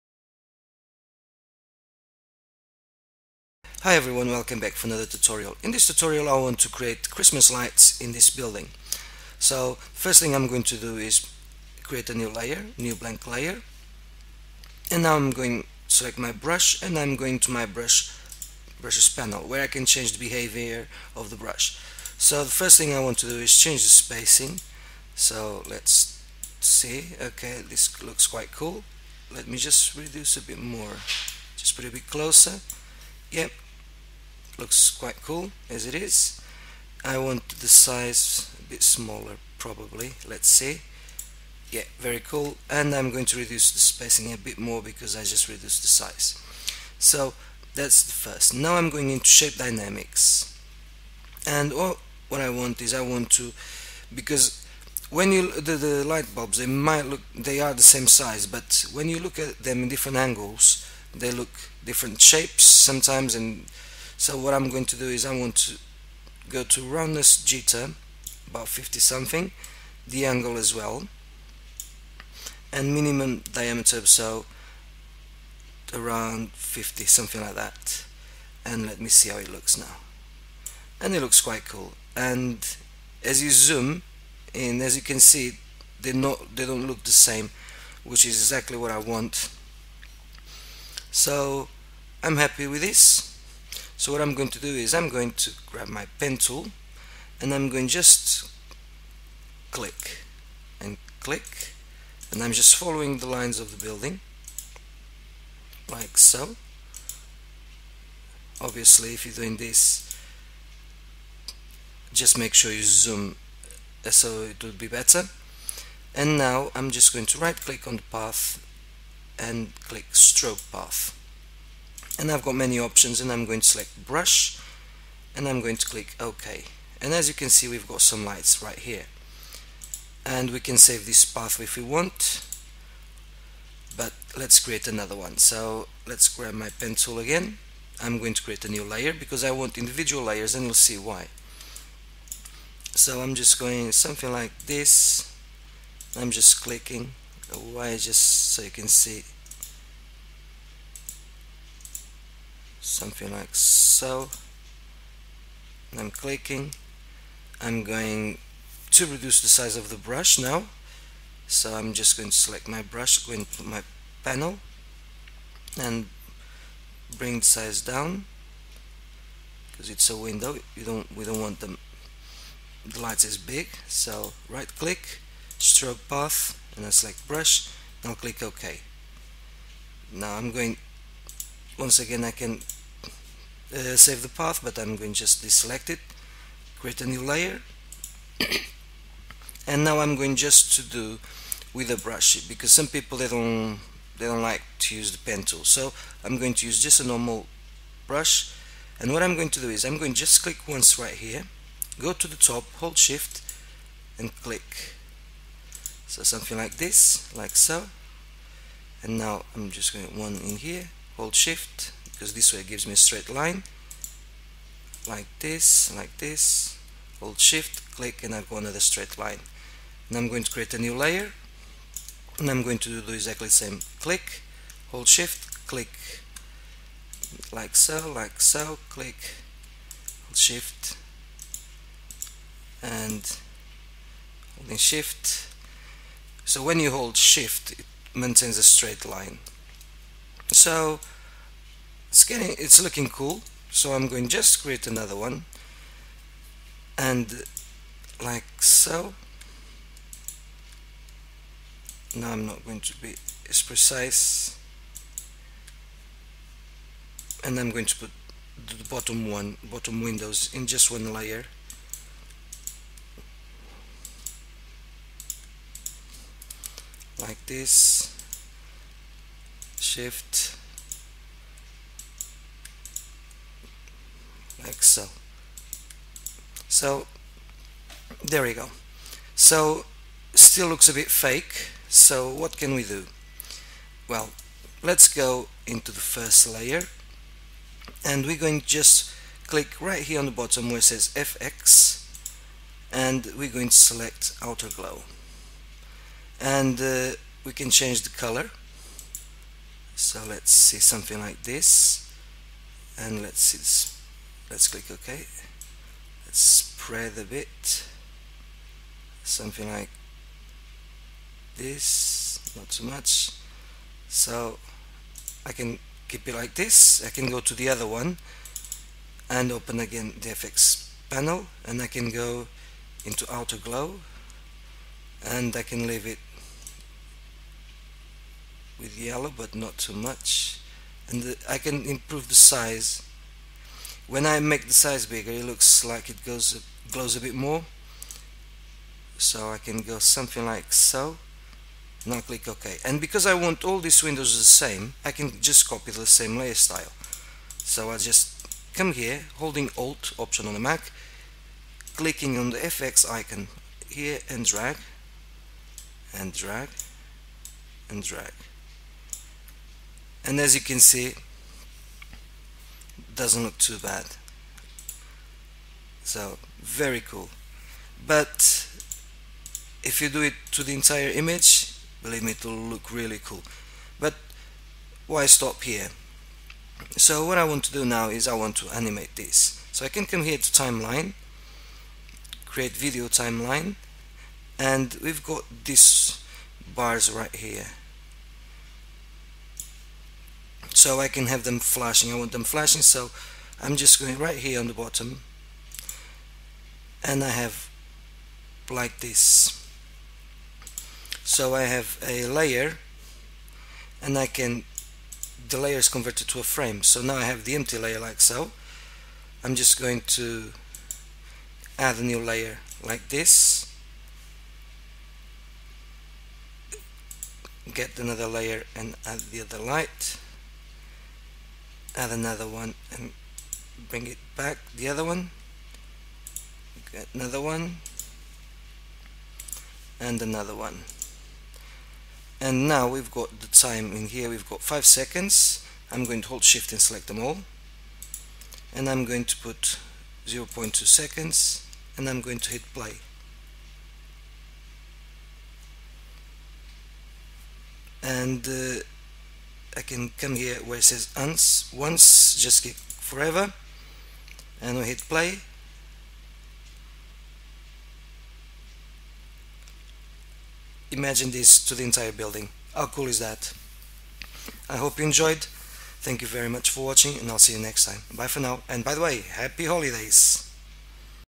Add Christmas lights to a building
Hi, in this tutorial I show you how to add Christmas lights to a building using Photoshop and also I will animate the lights.